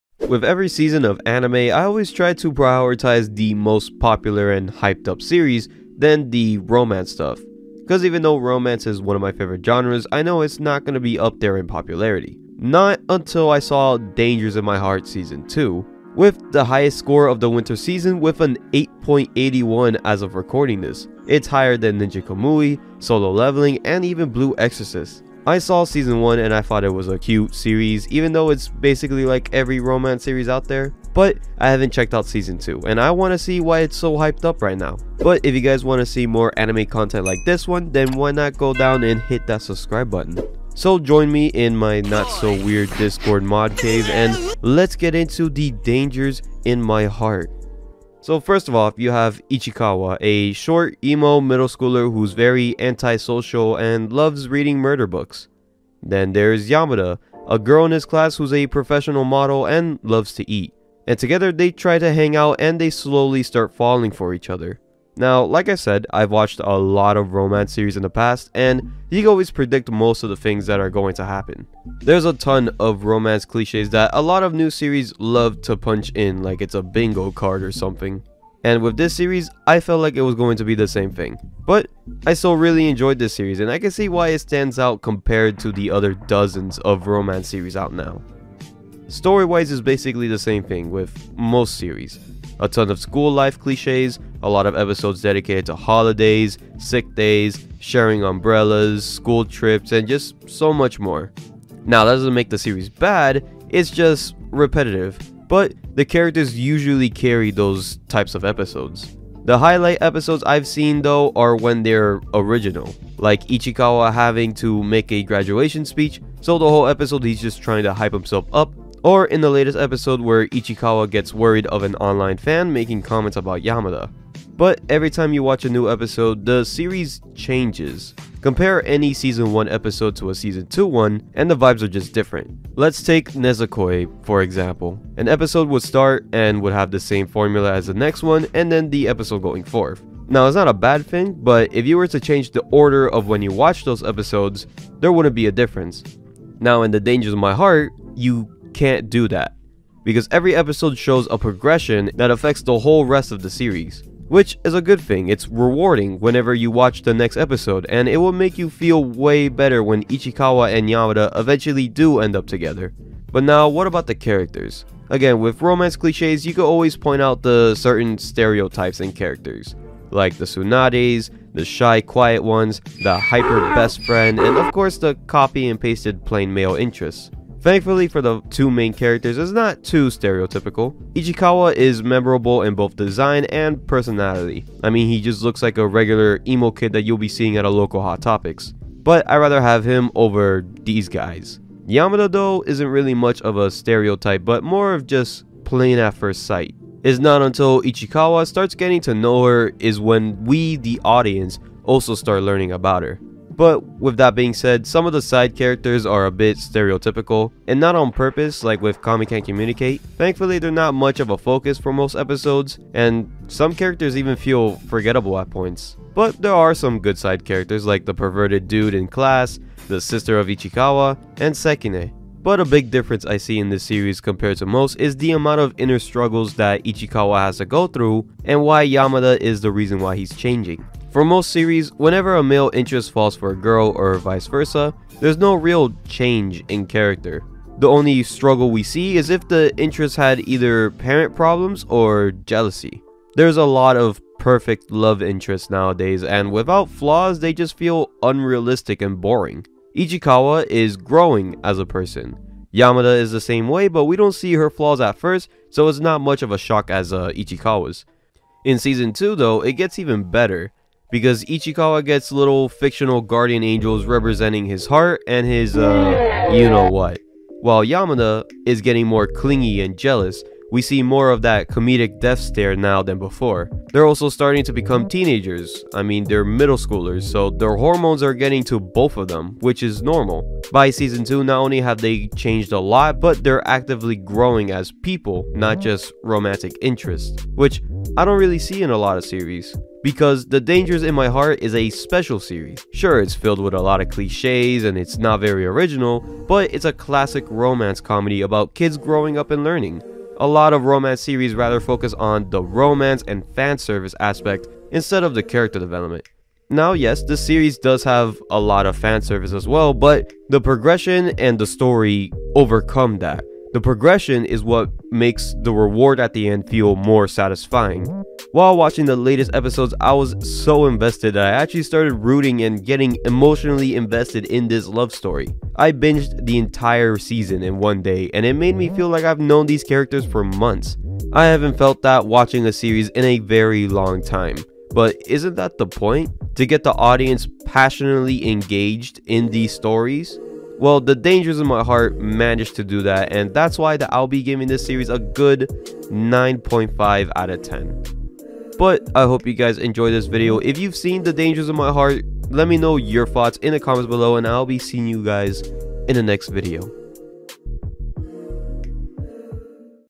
This Peak Romance Series is a Must See!!
The Dangers in My Heart is a New Romance Comedy Series about a boy named Ichikawa falling in love with a model in his class named Yamada. The show's season 2 has broken the internet!! Find out why by watching!!